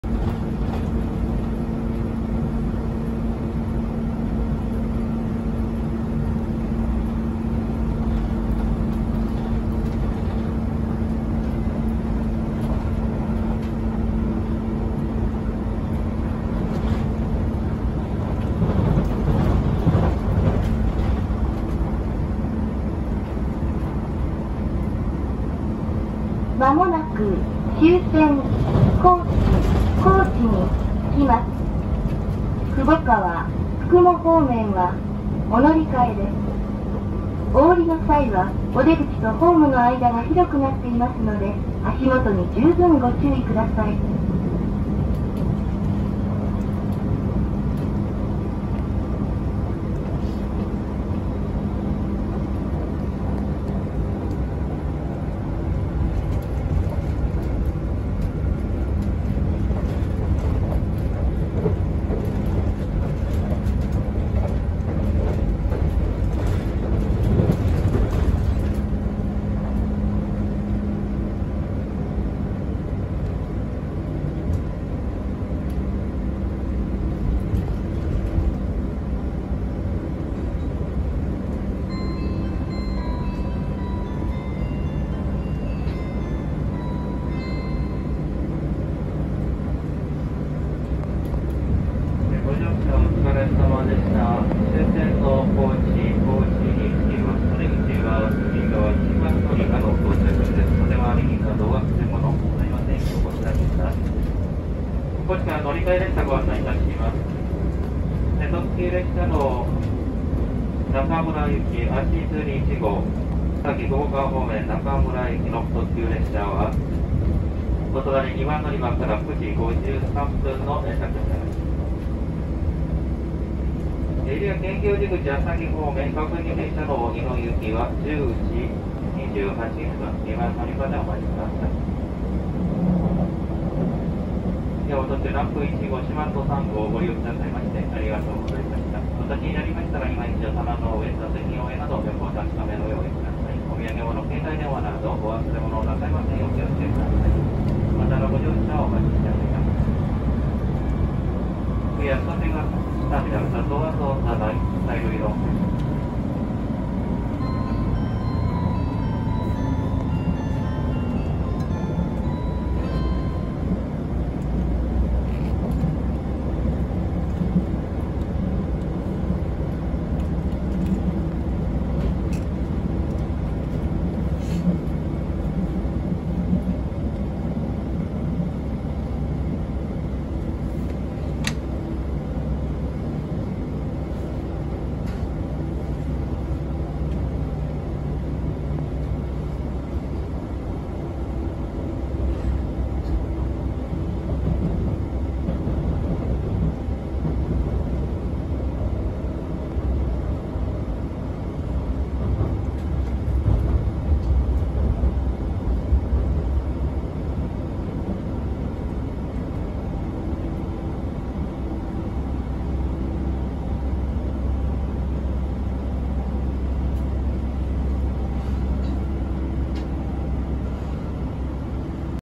【強烈なブレーキ！】N2000系 しまんと3号 高知駅到着前放送フル
高松から2時間あまり駆けてきた列車もまもなく終着です。それほど長くはない到着放送とブレーキ音をお楽しみください。